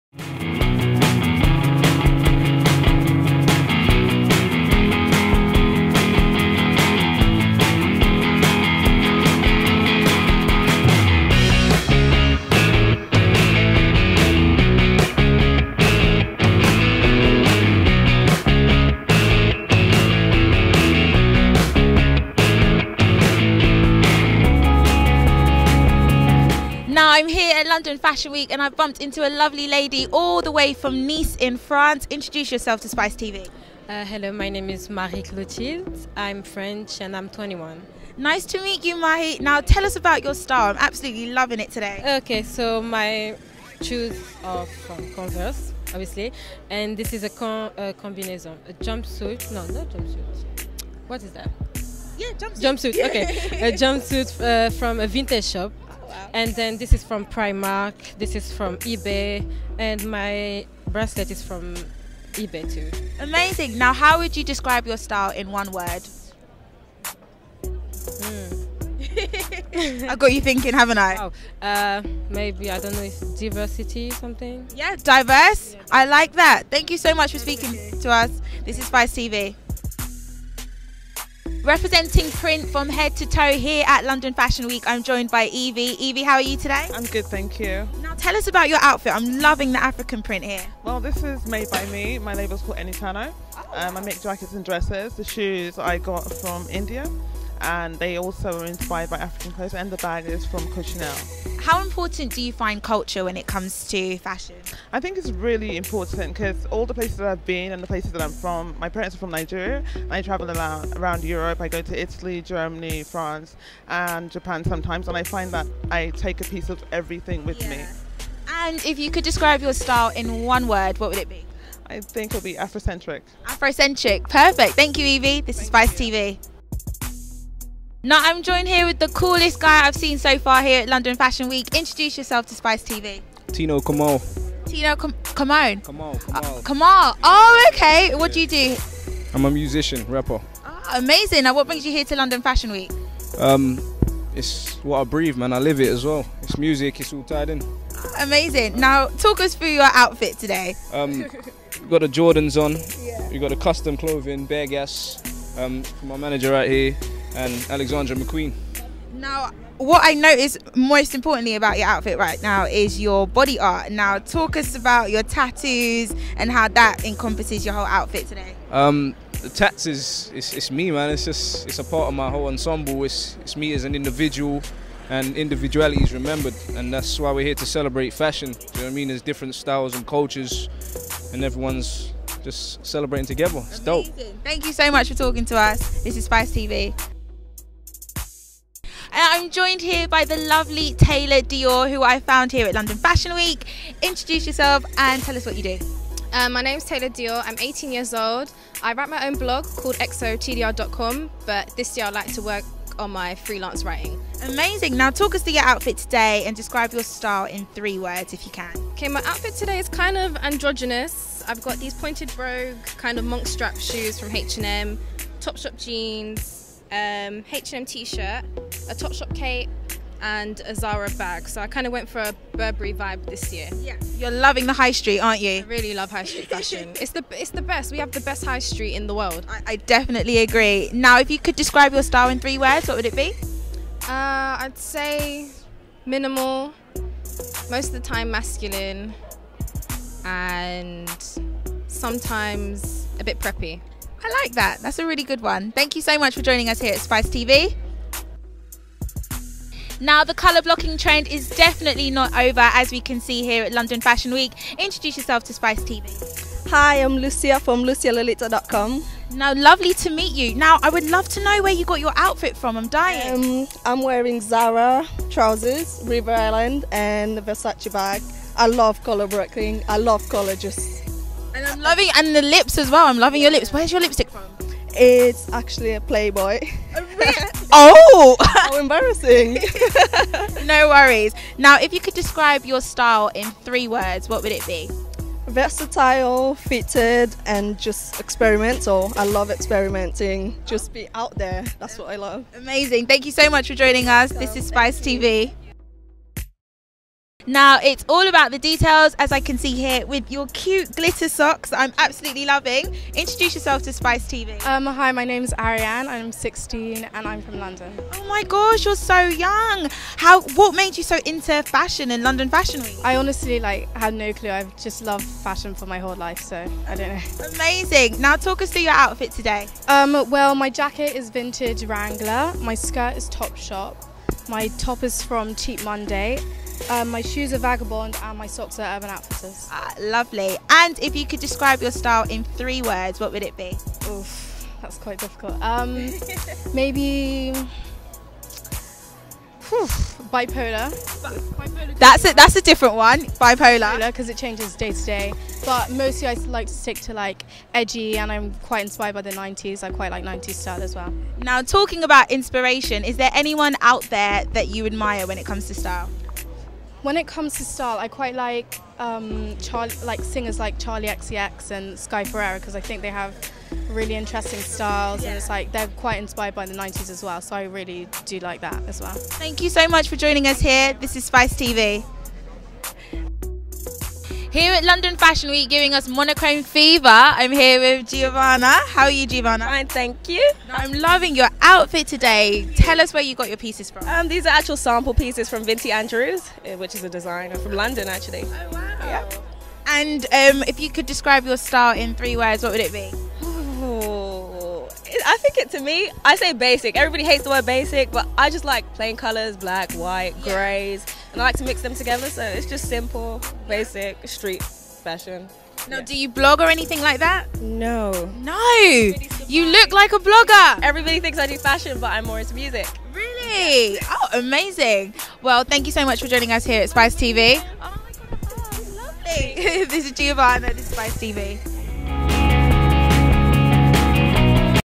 SPICE Street Style | London Fashion Week SS15 (1)
Follow SPICE TV as Mercedes Benson presents the best street style at London Fashion Week SS15. Watch all the best looks from celebs, stylists bloggers to social media stars in action outside the shows.